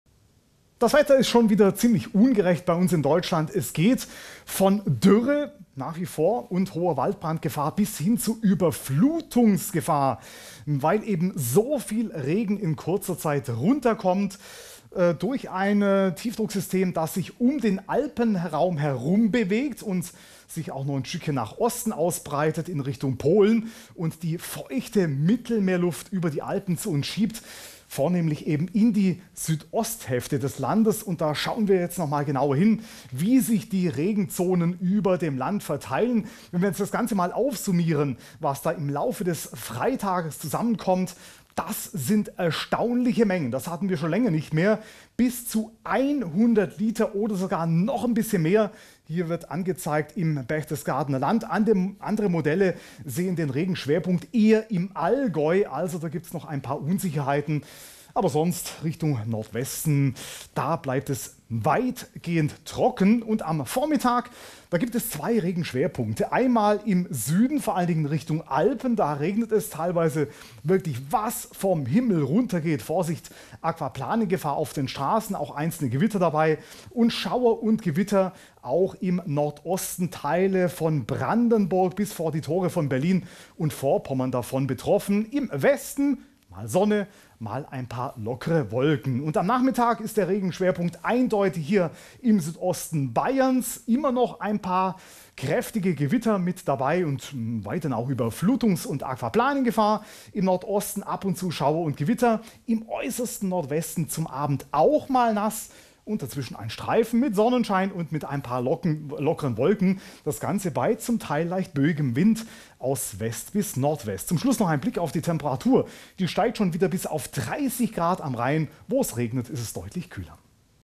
Wetter heute: Die aktuelle Vorhersage (19.08.2022)
Hier bekommst du die aktuelle Wettervorhersage für heute in Deutschland. Unsere wetter.com Meteorolog:innen Bernd Madlener, Anna Gröbel, Georg Haas, Corinna Borau, Andreas Machalica, Hartmut Mühlbauer, Alban Burster und Gernot Schütz präsentieren sie dir täglich.